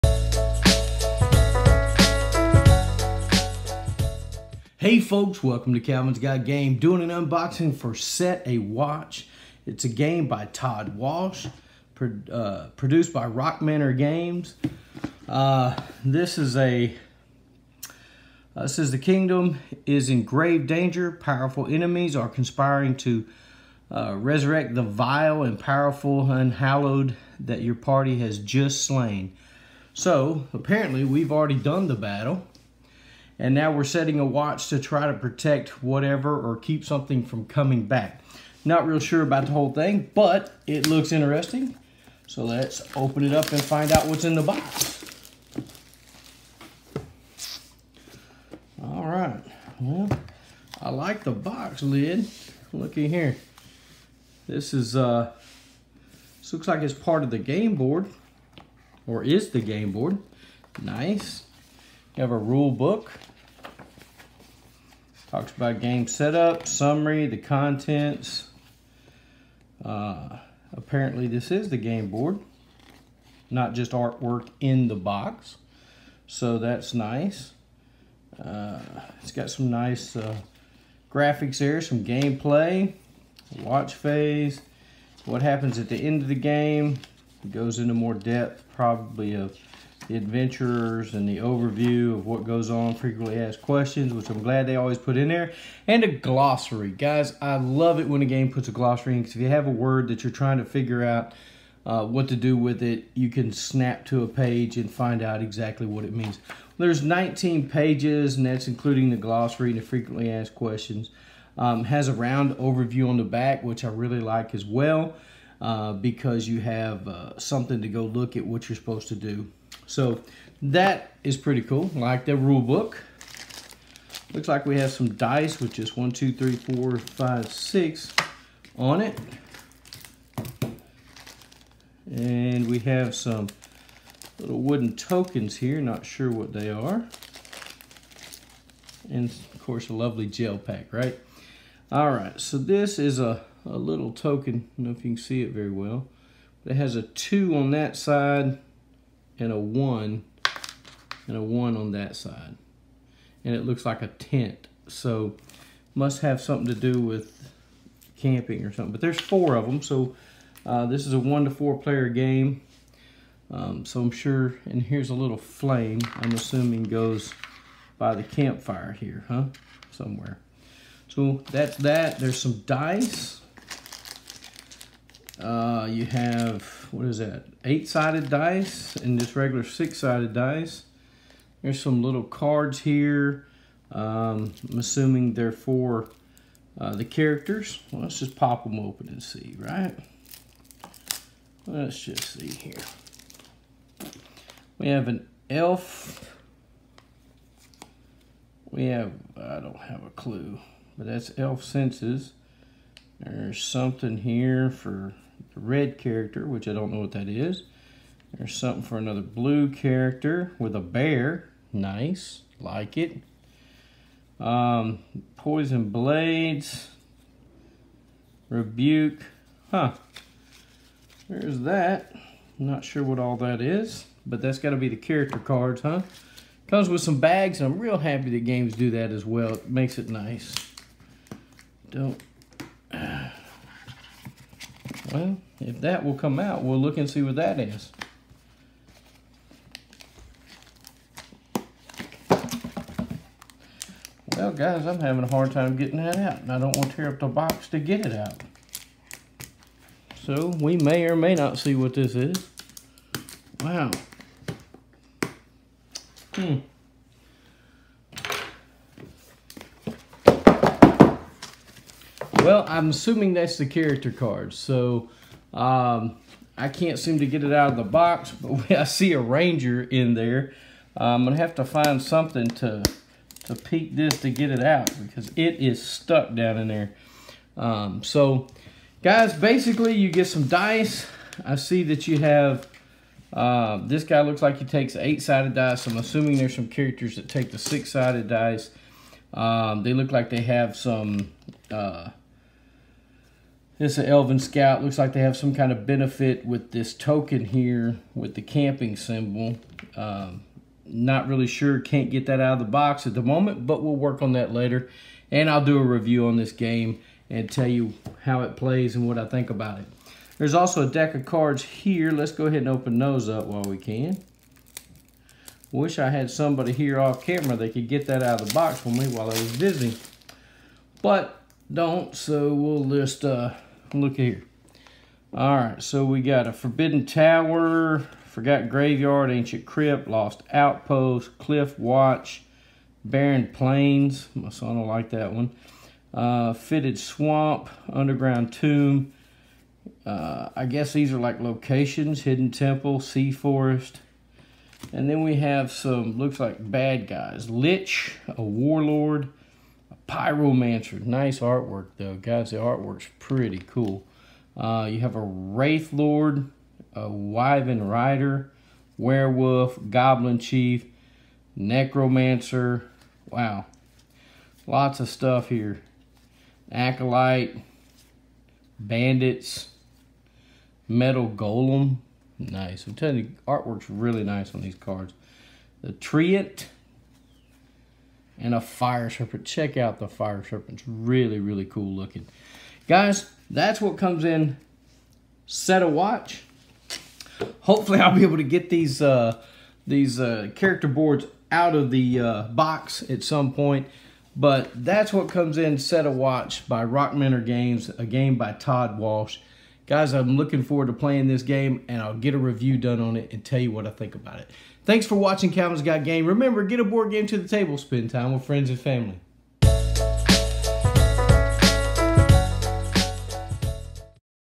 CGG: Set A Watch Unboxing
Set A Watch is a game where you have already completed the quest of beating the big bad boss, now the minions are coming to try to resurrect it. You must Set A Watch and keep the fire burning so you can see the hordes coming and defeat them.